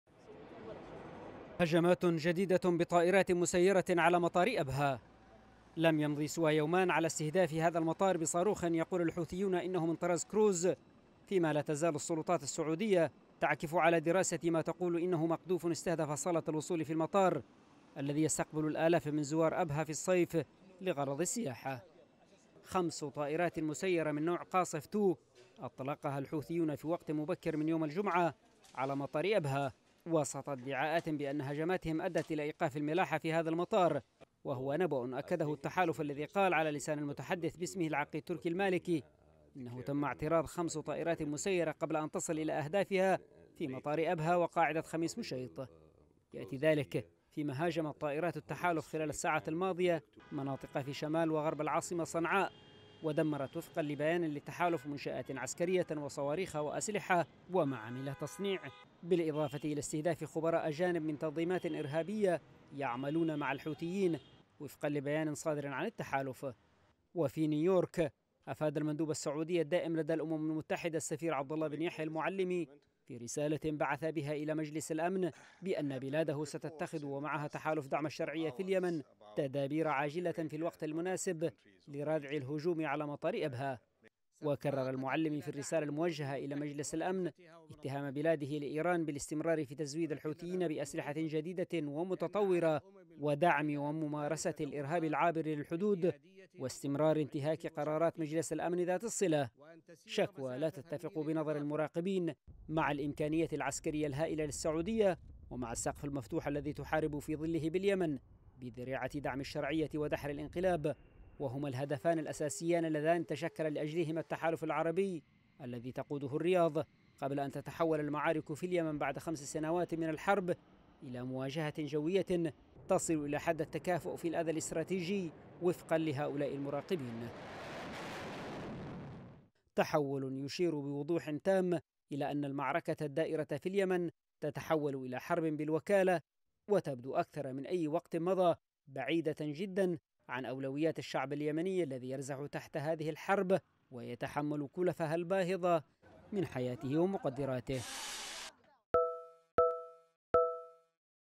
هجمات حوثية جديدة على مطار أبها والتحالف يعلن إسقاط خمس طائرات مسيرة | تقرير: ياسين التميمي
توعدت الرياض، باتخاذ تدابير عاجلة لردع ومحاسبة المسؤول عن الهجوم على مطار أبها، جنوبي السعودية.
وأفاد مندوب السعودية الدائم لدى الأمم المتحدة عبد الله المعلمي، في رسالة إلى مجلس الأمن الدولي أن بلاده ودول التحالف ستتخذ تدابير عاجلة، في الوقت المناسب لردع هجمات مليشيا الحوثي...